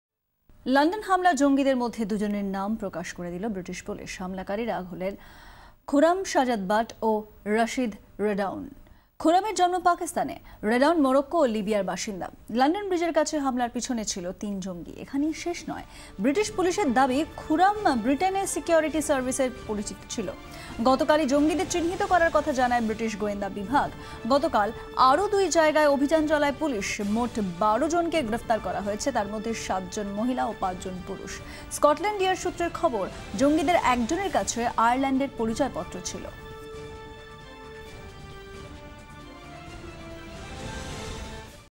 British Police Name 2 Attackers From Terrorist Attack In London
London Police named two attackers involved in Saturday's violent rampage on and around London.To satisfy the demands of high-rise favourites of Bengali audiences, Zee 24 Ghanta brings all the latest headlines and news stories.